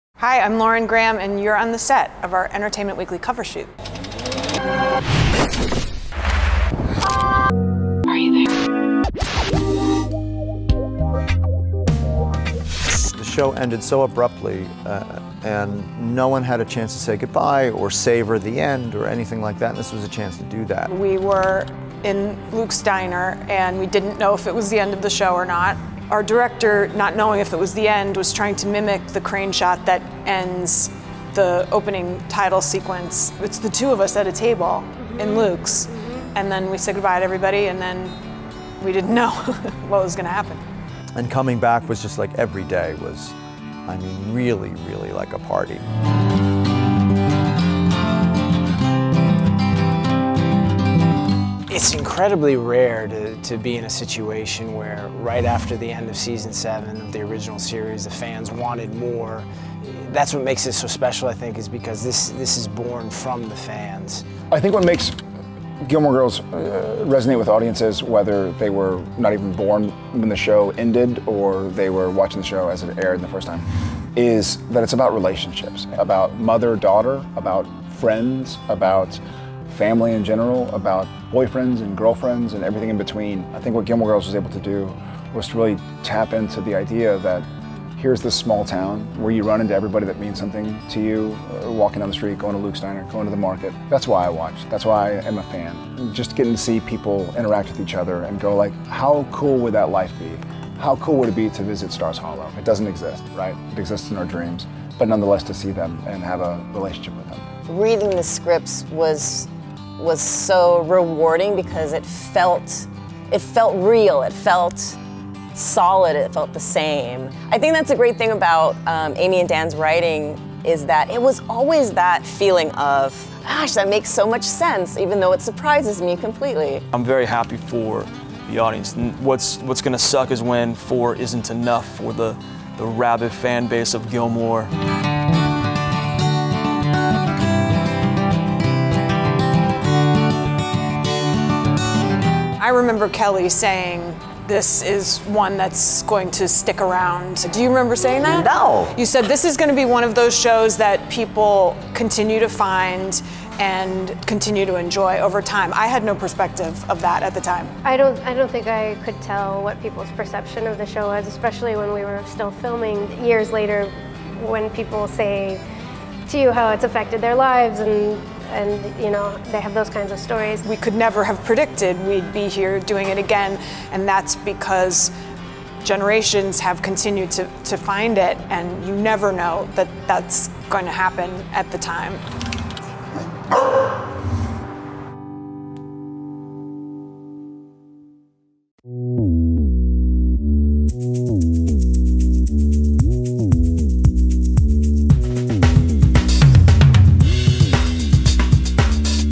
Gilmore Girls: Alexis Bledel, Lauren Graham & More On New Show | Cover Shoot | Entertainment Weekly
Gilmore Girls stars Alexis Bledel, Lauren Graham, Jared Padalecki, Milo Ventimiglia, Matt Czuchry, Scott Patterson, Kelly Bishop, and Keiko Agena talk about the revival of the show, Netflix's 'Gilmore Girls: A Year In The Life', and share secrets from the original series.